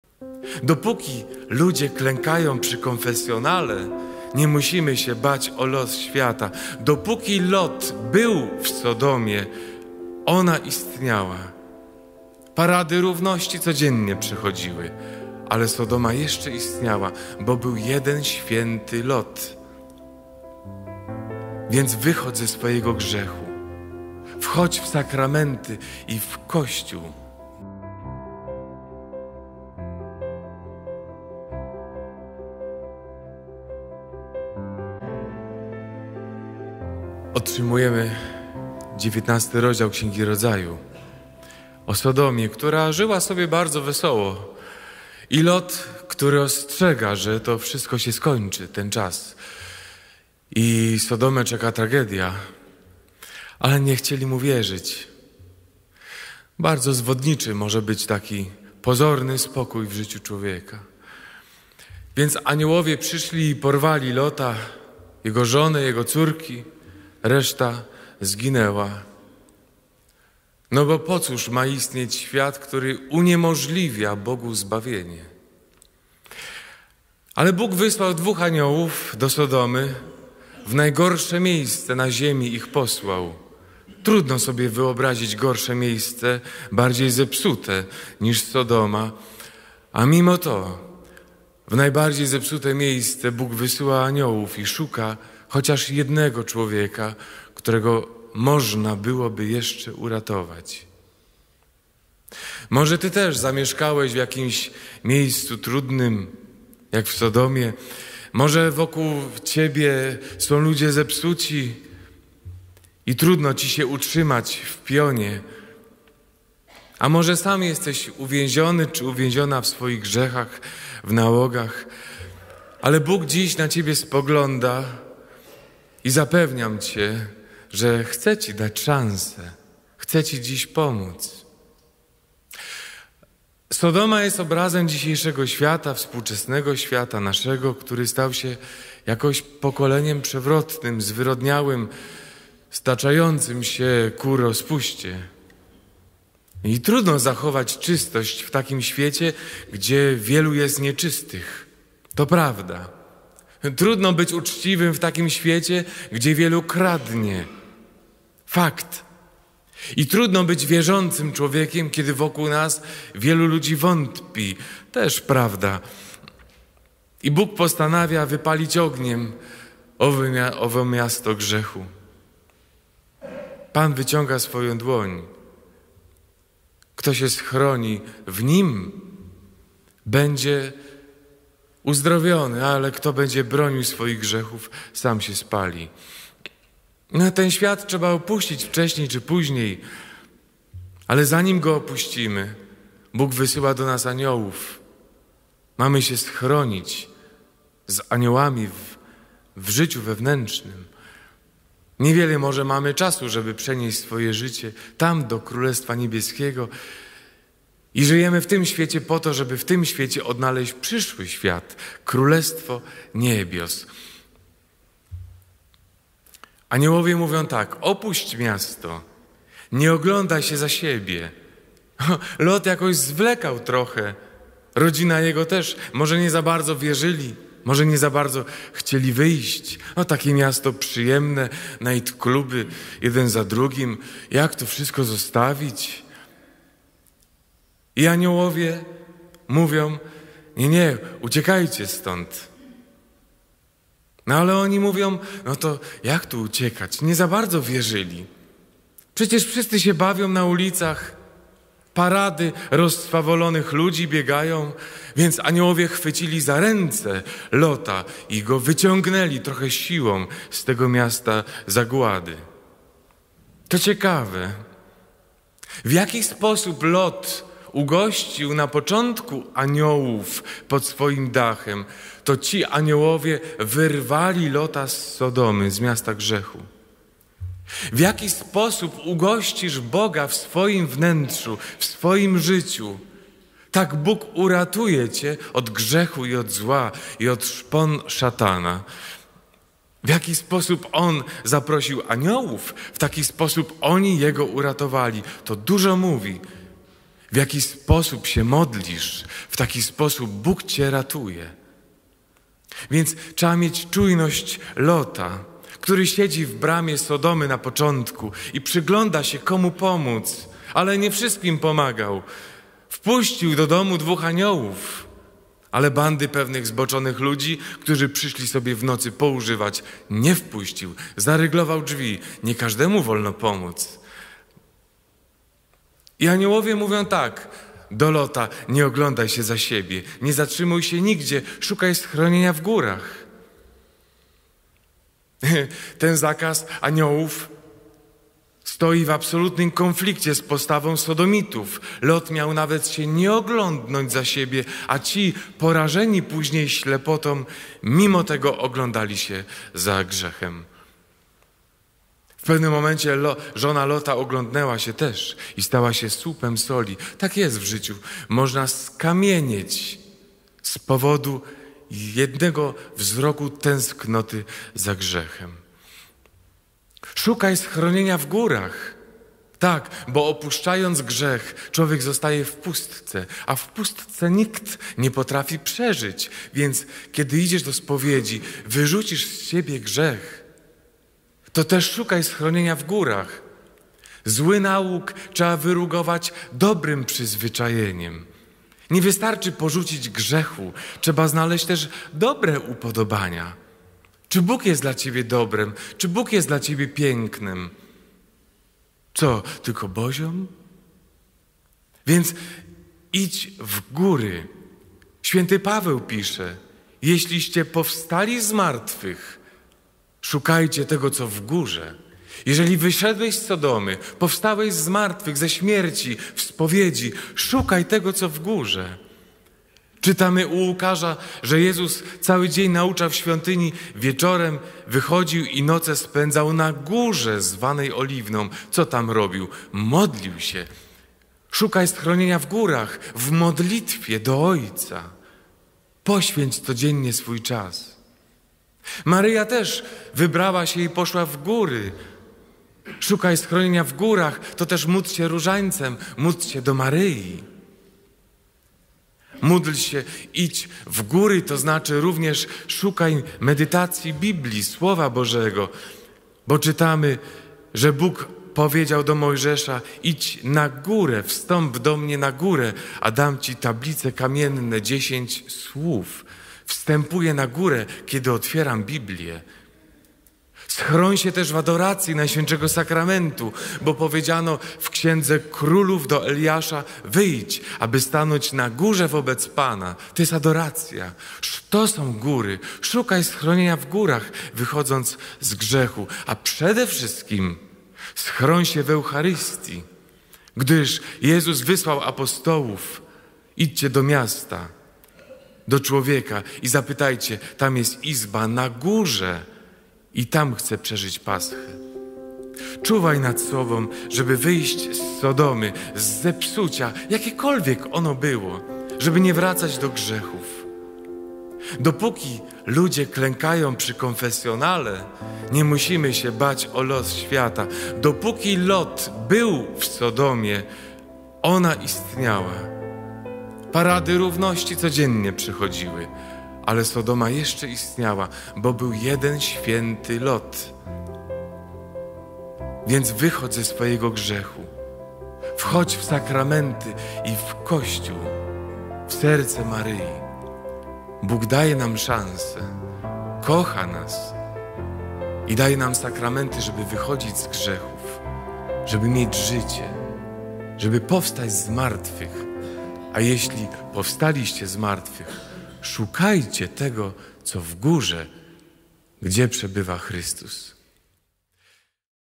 23. "Uciekaj z Sodomy" - o. Marcin Ciechanowski [Jasna Góra]
Spis
0:00     Intro
0:36     Życie w Sodomie
1:41     Bóg daje Tobie szansę
3:35     Rada Aniołów
4:23     Jak się modlisz - tak Bóg Cię ratuje
5:02     Nie każdemu można pomóc
6:06     Schroń się w górach - co to znaczy?
8:22     Opuść Sodomę - opuść grzech

Homilia O zniszczeniu Sodomy   Rdz 19,15 29
głosi: o. Marcin Ciechanowski
Jasna Góra 04.07.AD 2023

* Jedynym celem tego kanału jest ewangelizacja. Ten kanał nie zarabia na siebie. Reklamy pojawiają się z racji polityki YouTube.